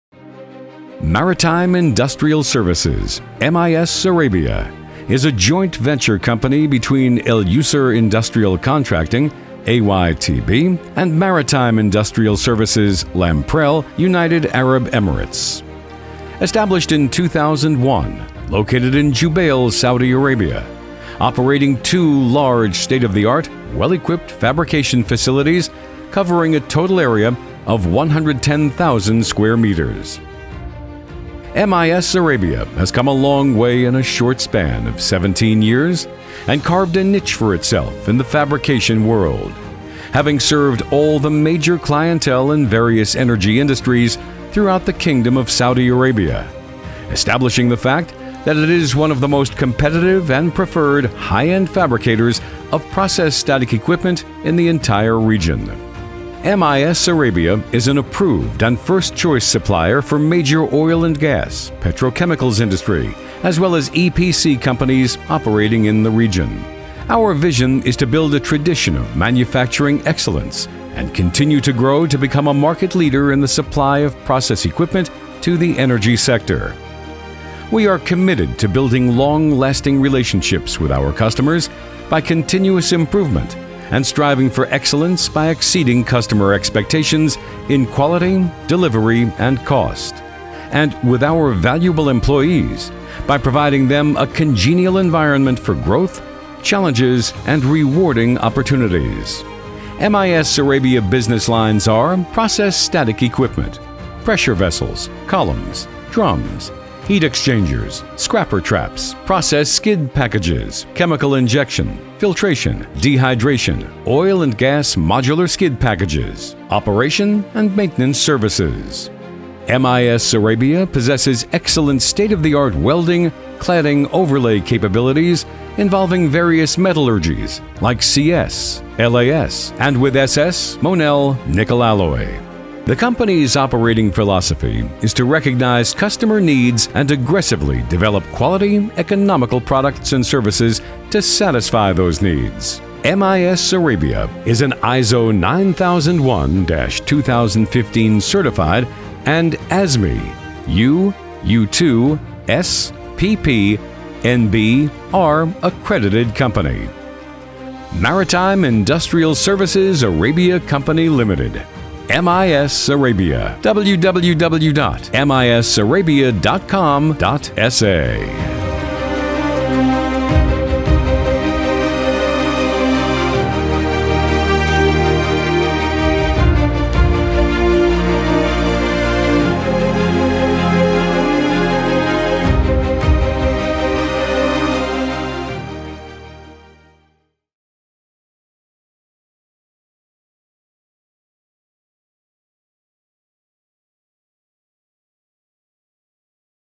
Maritime Industrial Services M. I. S. Arabia
Maritime Industrial Services M. I. S. Arabia, is a joint venture company between Al Yusr Industrial Contracting A.Y.T.B. and Maritime Industrial Services (Lamprell) United Arab Emirates. Established in 2001, located in Jubail, Saudi Arabia.